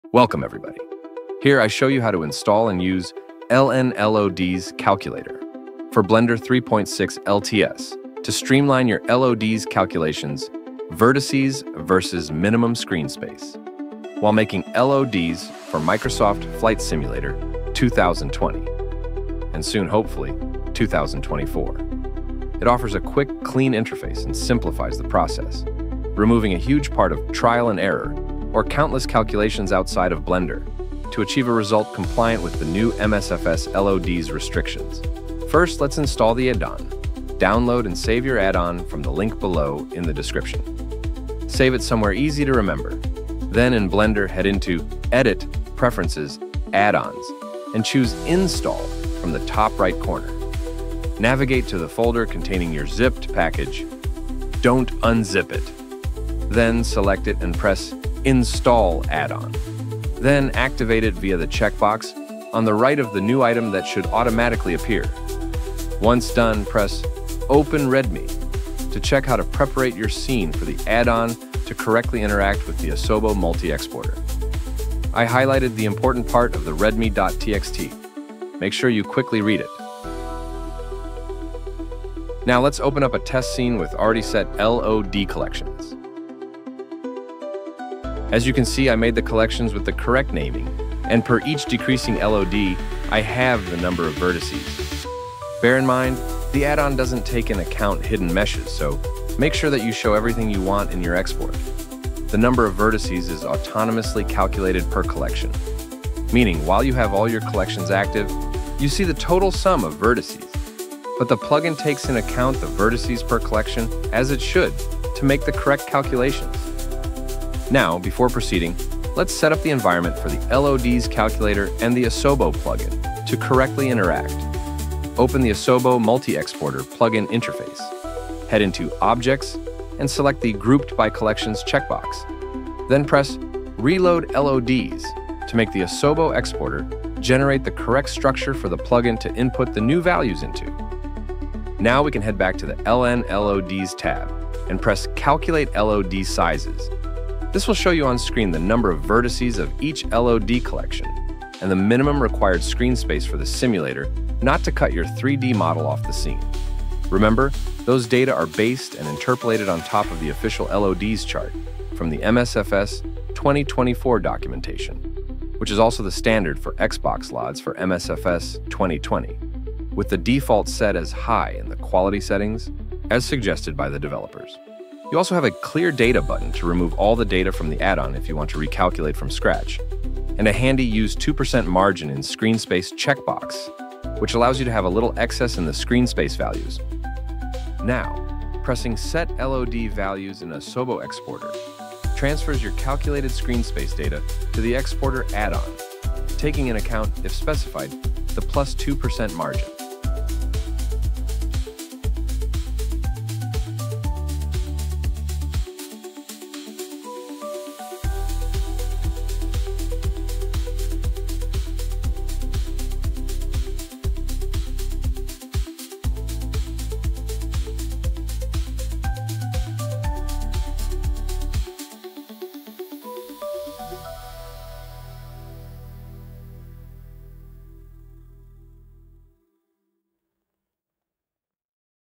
LODs Plugin for MSFS in Blender 3.6
Hey everyone! This new plugin, is here to streamline the process of LODs creation by allowing the user to automate calculations of Vertex Count Vs. Minimum Screen Space straight into blender. This interfaces seamlessly with the Asobo exporter (for MSFS2020 for now) allowing you to send the calculated screen size straight to the addon without having to manually calculate, use trial and error, or use external apps. Remember to report any bug, issue or idea straight into my Discord Server (link below)